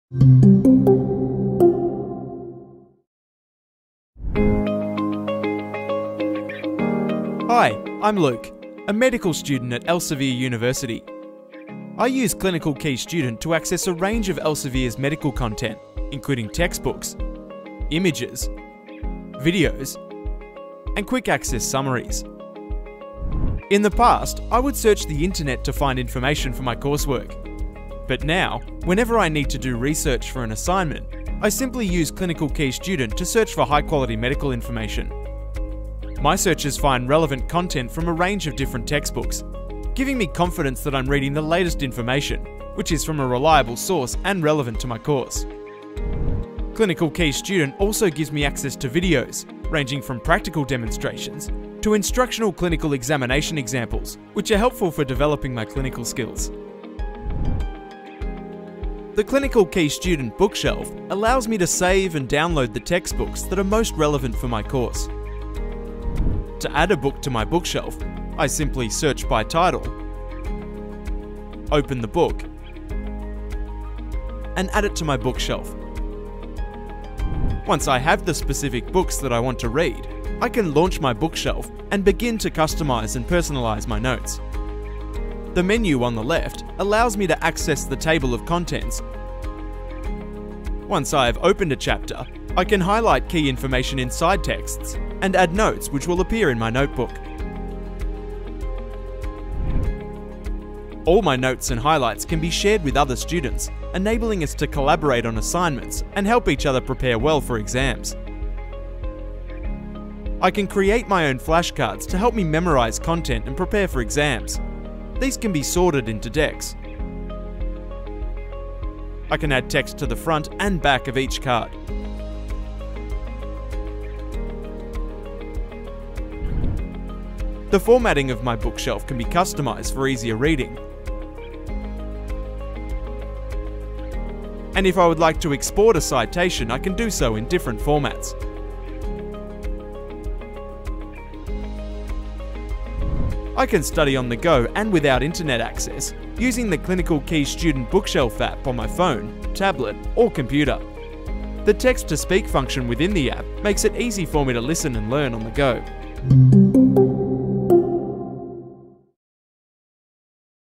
ClinicalKey Student Medicine Foundation - Compiled Student Training Video
A compiled student training video focusing on the Search and Bookshelf use-cases of ClinicalKey Student Medicine Foundation.